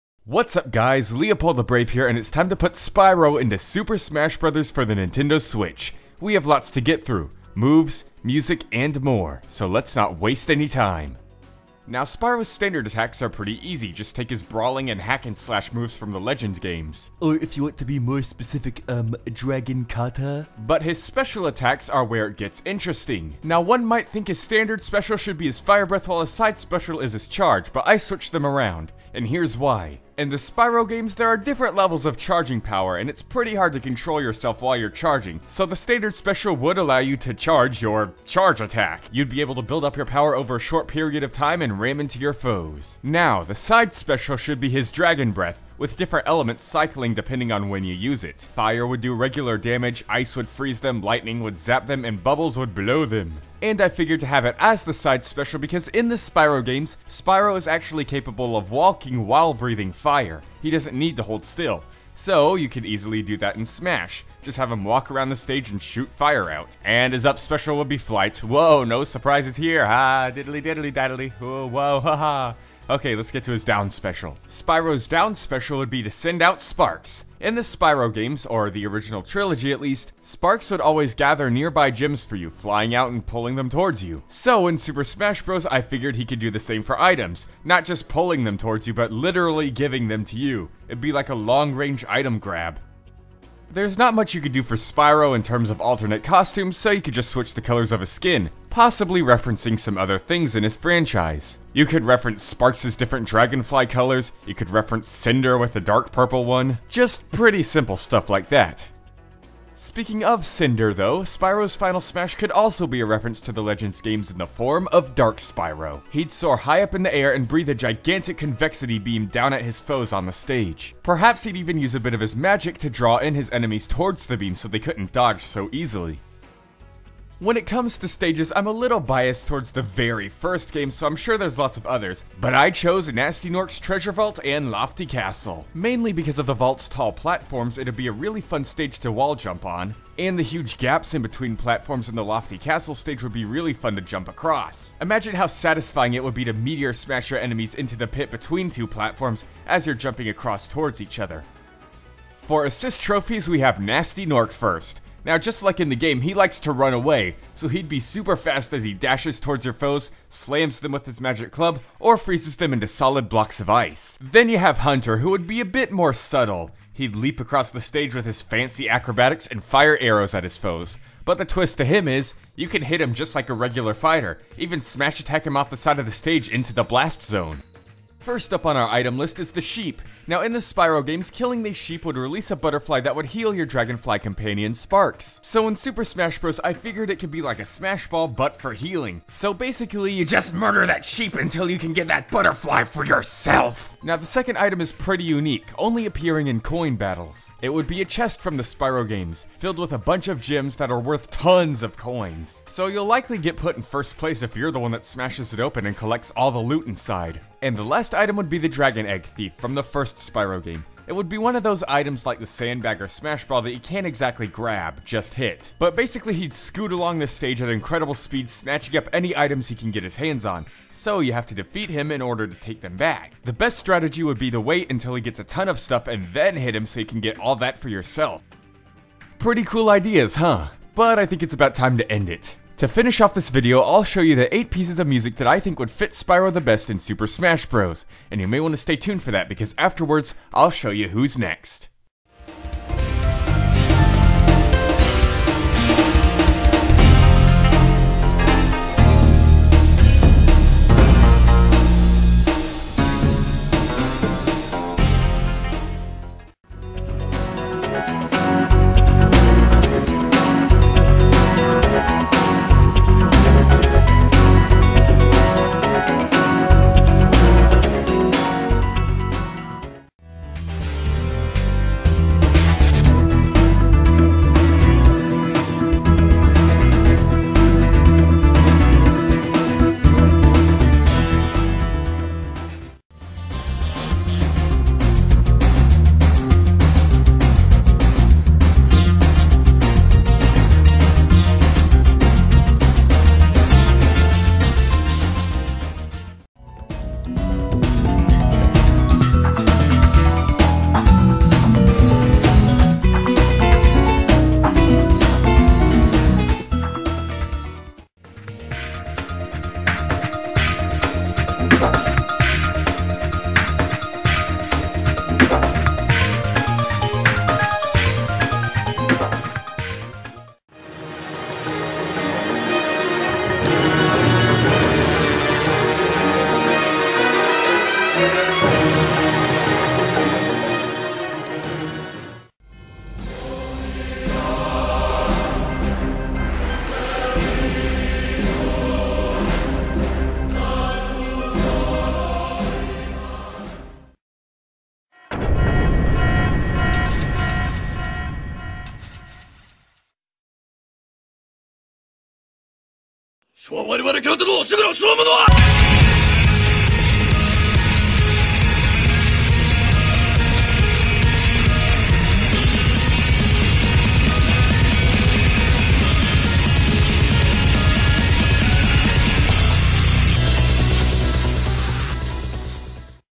Spyro - SUPER SMASH BROS. SWITCH IDEAS
THE SPYRO RENDER USED IN THIS VIDEO AND THUMBNAIL WAS CREATED BY - HARRY DARLINGTON
How do you think Spyro would play?